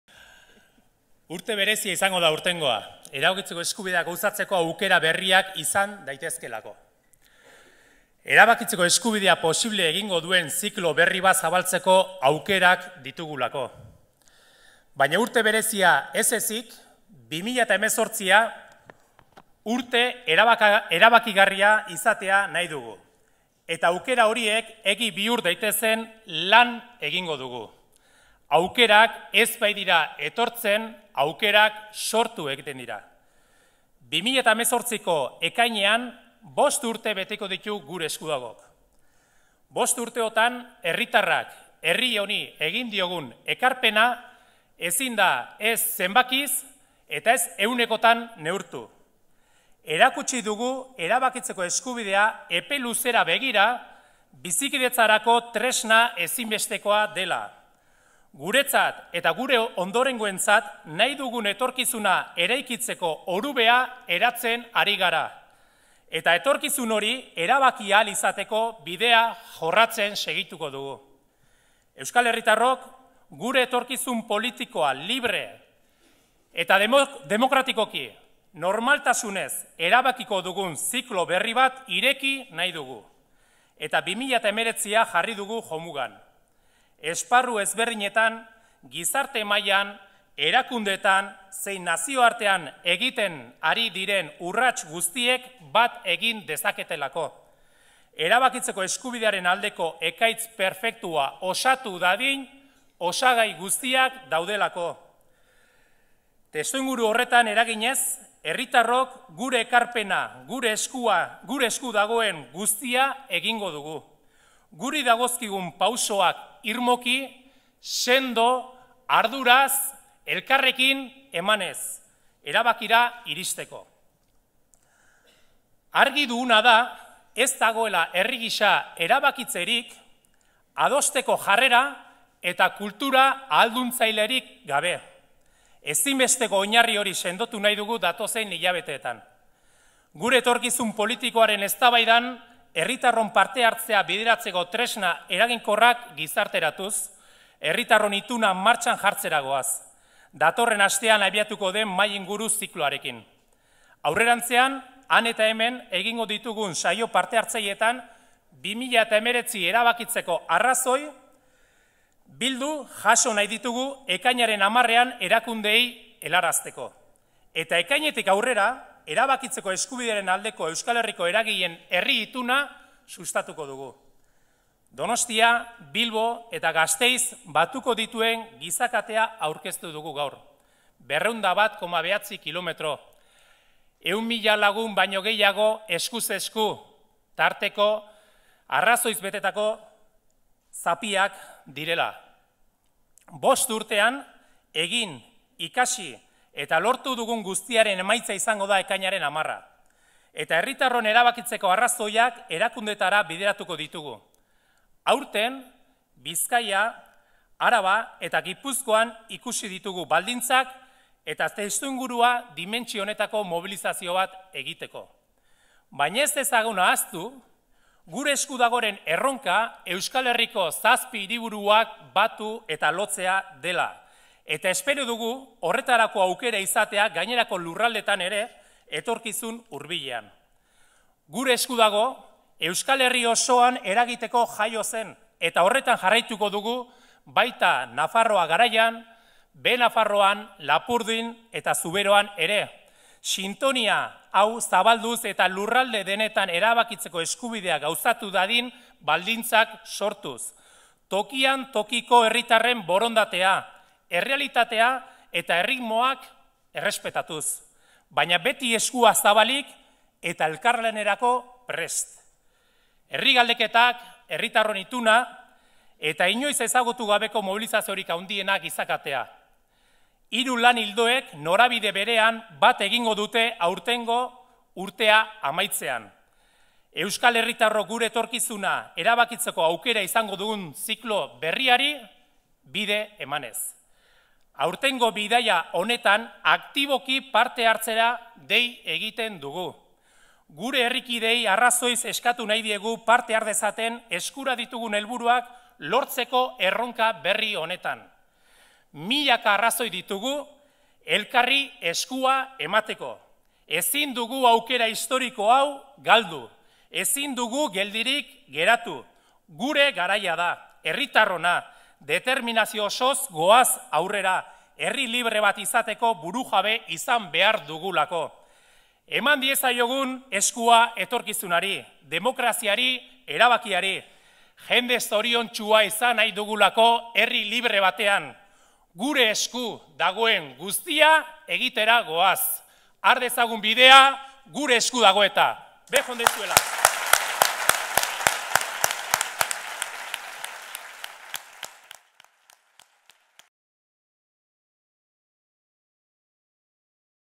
Angel Oiarbide: "Ezin dugu aukera historiko hau galdu"
Eureka Zientzia Museoa. Giza katearen aurkezpena. Gure Esku Dago.